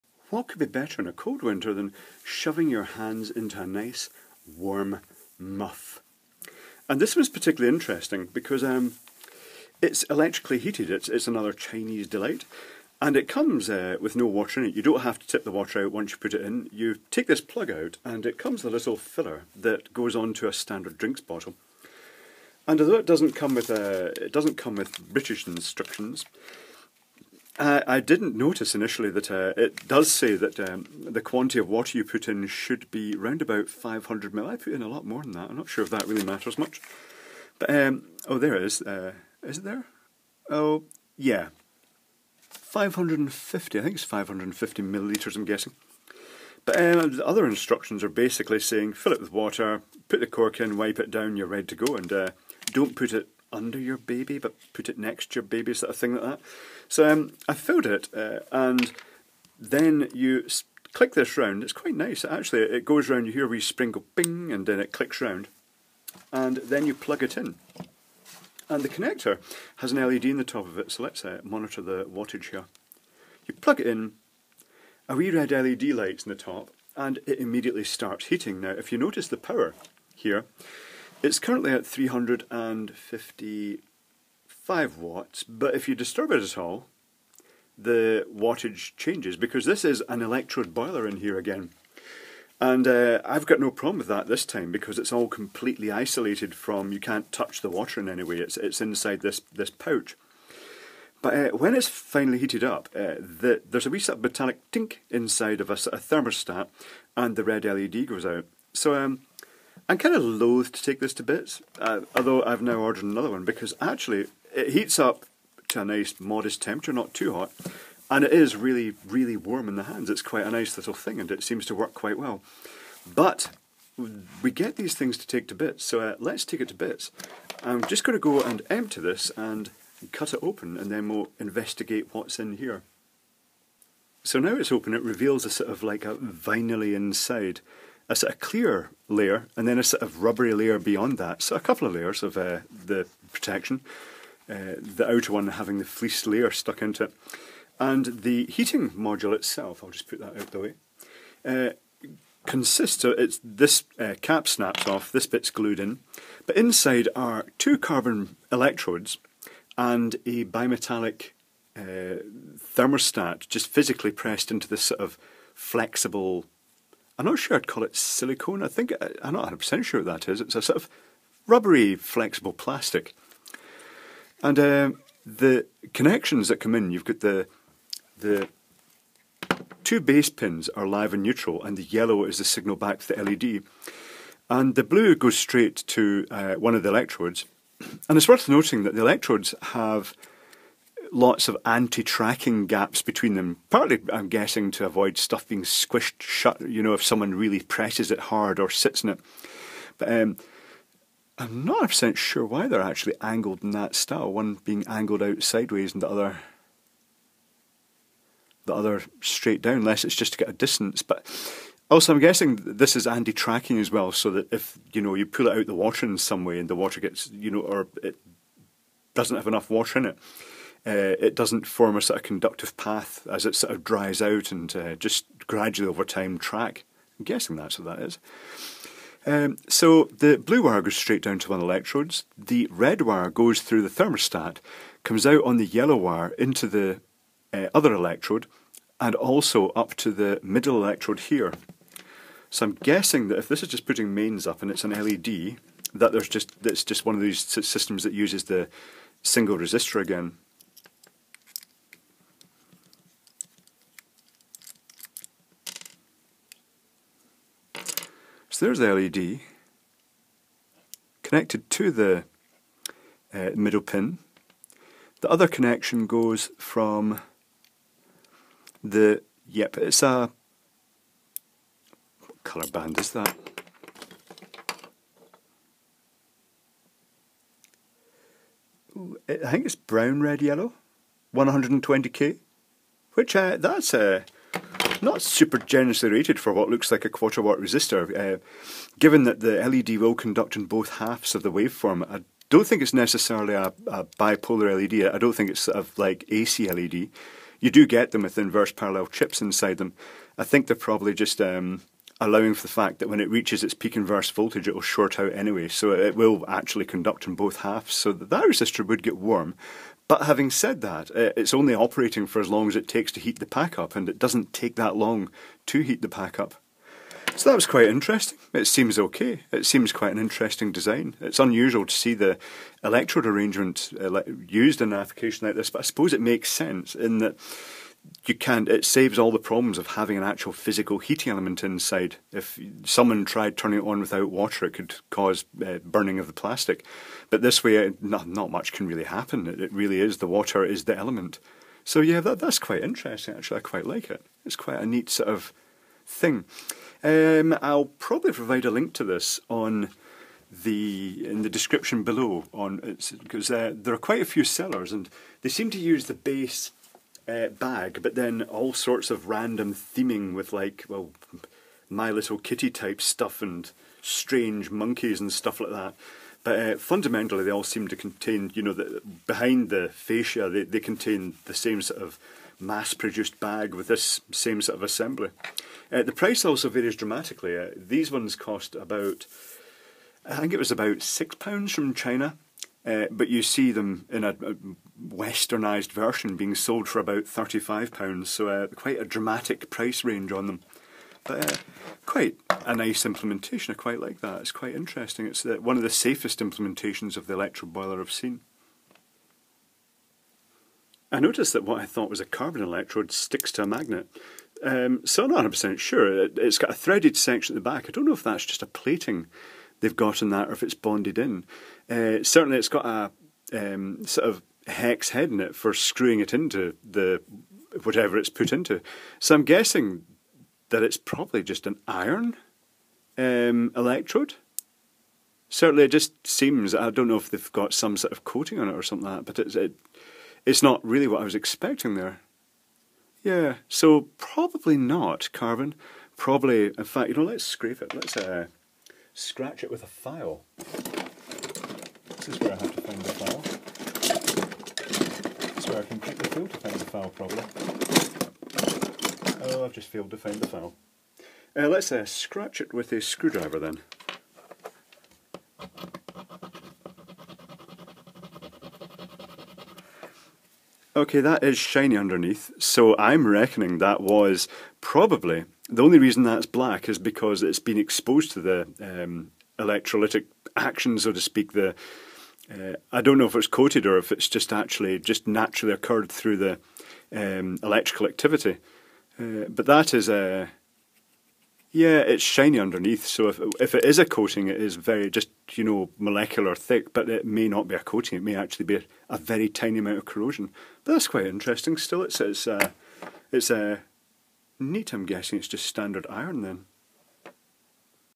Teardown of an electrode heated muff / hot water bottle.
An electrode water heating device that is relatively safe for a change (although it relies on a single thermal switch).  This one has the electrified water inside a sealed two layer plastic/vinyl bag with a thermal switch to turn it off once heated.  I'm not sure what would happen if the thermal switch failed.  The bag might swell up with steam to the point the electrodes were clear of the water, or it might just burst.

The listing I bought this from was as follows, noting that it is specified as a 220V unit, so it will take longer to heat on 110V.